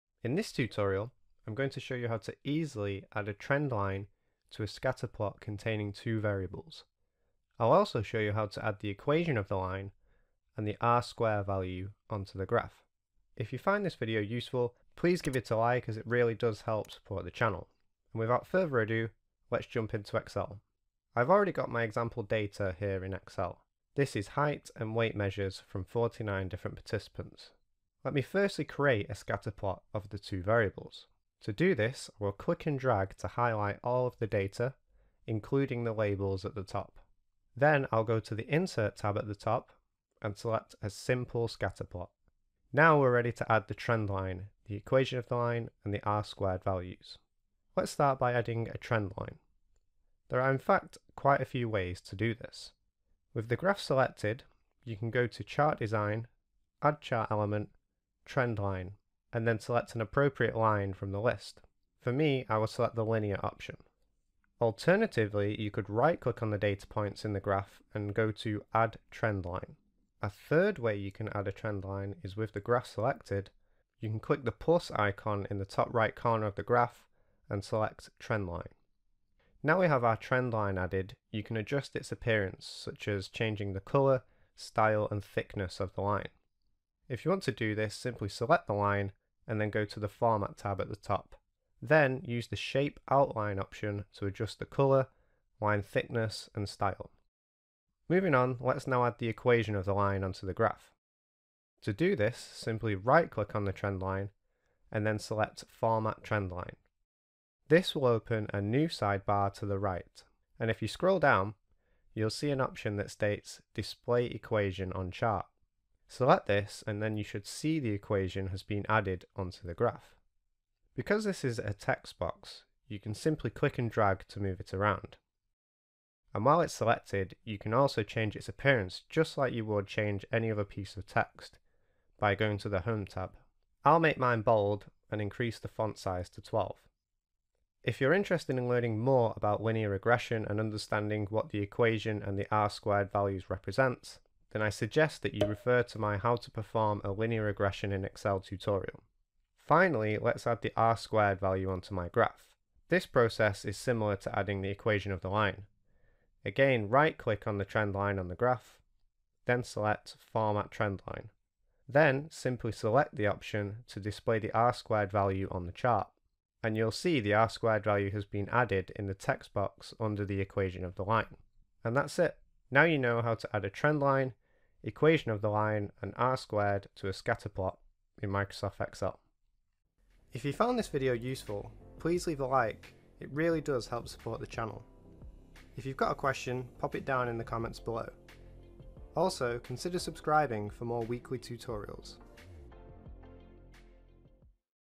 Adding The Trendline, Equation And R2 In Excel
In this tutorial, I’m going to show you how to easily add a trendline, equation of the line and R2 value to a scatter plot in Microsoft Excel.

VIDEO CHAPTERS
00:00 Intro
00:21 My example data
00:28 Creating a scatter plot
00:47 How to add a trendline
01:37 How to add the equation of the line
02:28 How to add the R2
02:51 Wrapping up

Software (Microsoft Excel 365 ProPlus)